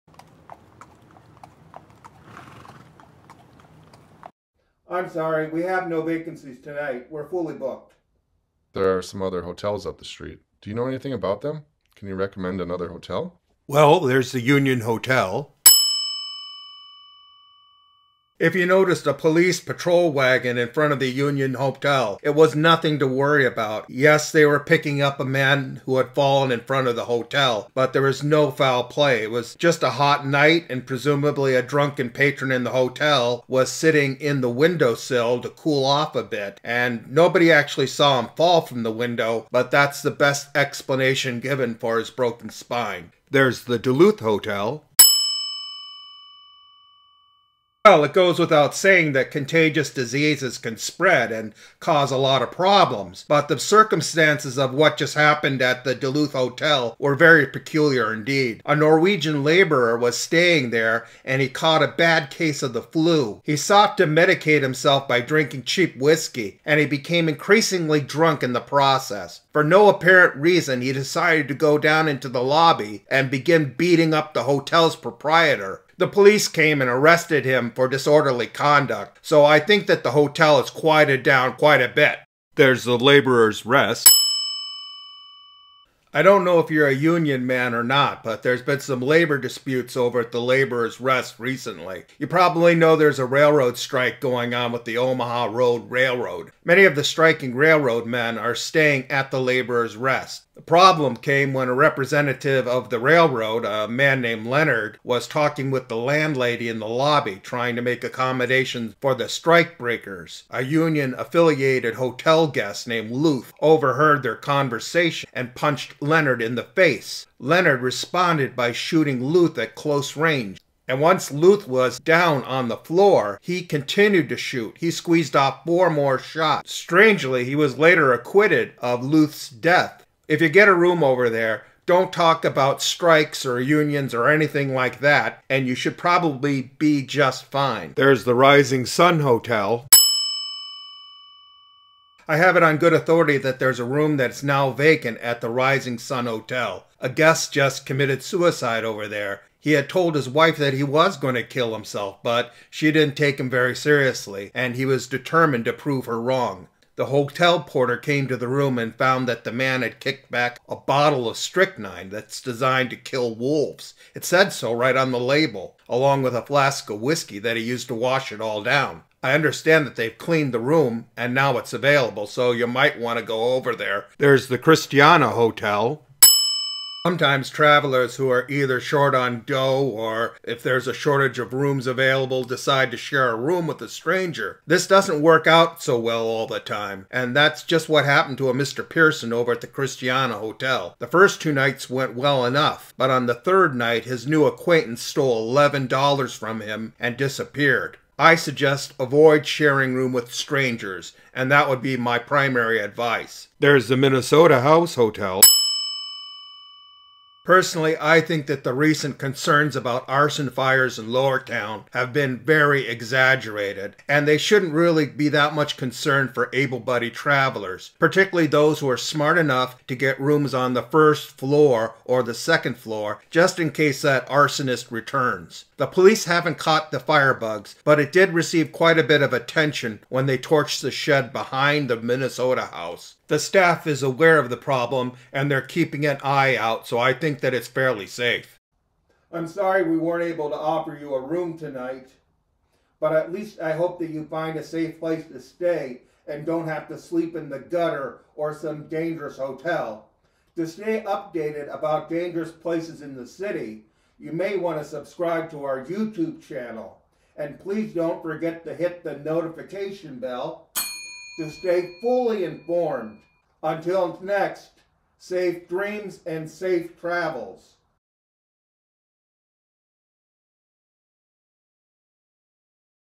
Hotels along Wall Street (formerly Rosabel Street) in downtown St. Paul, Minnesota in the 1890's.
Episode 7 of Forgotten St. Paul, Minnesota Hotels features lodging opportunities located along Wall Street in the 1890’s.  At this time Wall Street was named Rosabel Street.  In this history video, visitors are cautioned about drinking too much and sitting on hotel windowsills at the Union Hotel in the warmer weather.  Although, at the time, it may have seemed like a reasonable way to cool off in the summer months, it can have fatal results.  Drinking excessively is not a good remedy for recuperating from the flu as guest at the Duluth Hotel discovered after his arrest.  There was significant unrest at the Laborers’ Rest lodging house resulting in a highly publicized murder in the lobby.  A guest at the Rising Sun Hotel did not rise the next morning due to drinking wolf-killer.  Saving money on lodging by sharing a room at the Christiana Hotel does not always pay off for honest guests.  Although there has been several arsons in Lowertown, firebugs only burned down the barn behind the Minnesota Hotel and no guests sleeping in the hotel itself were inconvenienced.